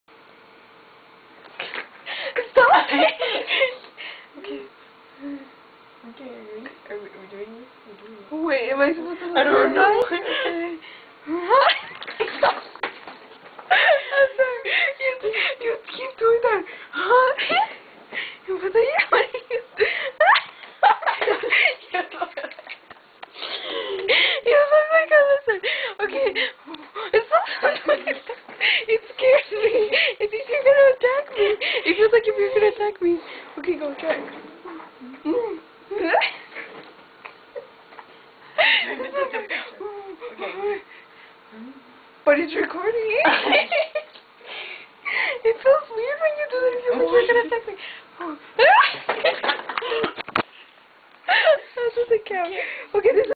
Lizard Eye Photo
fer and i were trying to take a photo where i was like...a monster and she was scared and we started laughing. so we meade a video of what made her laugh. lol.